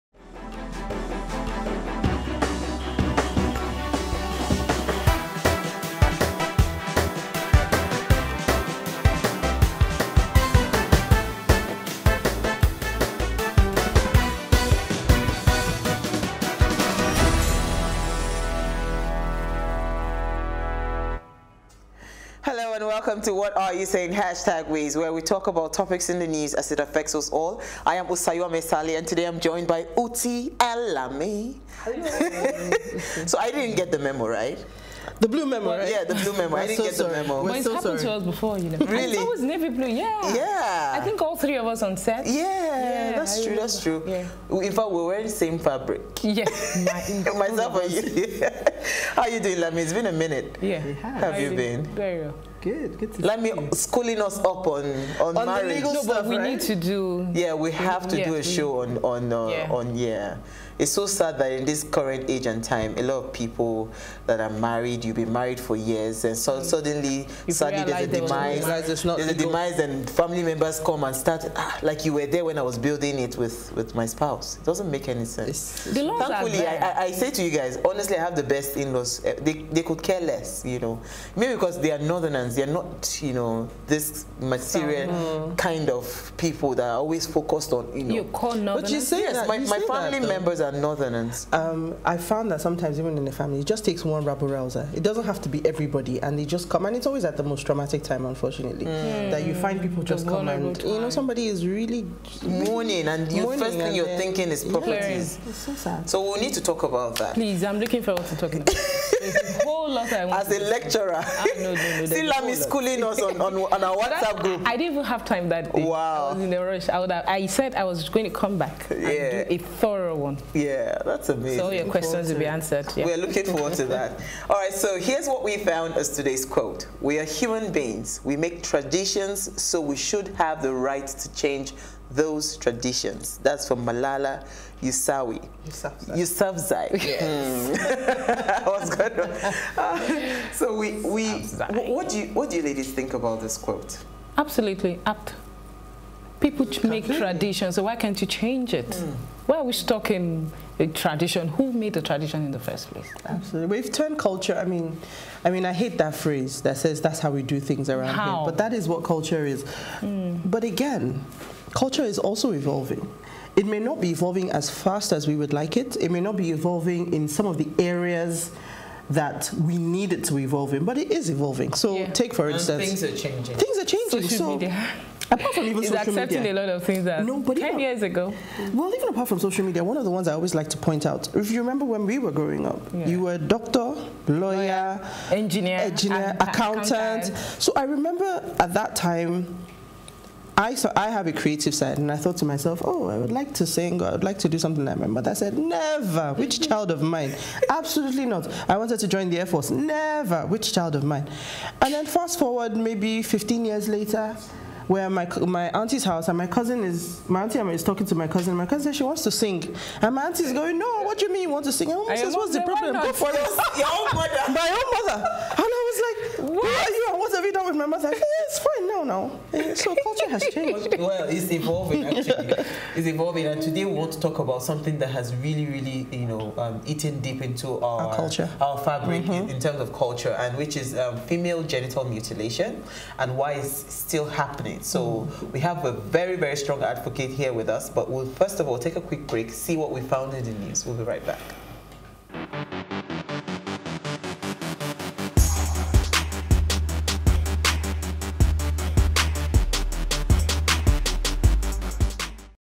Humans Made Traditions Therefore We Can Change Them
"We are human beings, we make the traditions so we should have the right to change those traditions" - Malala Yousafzai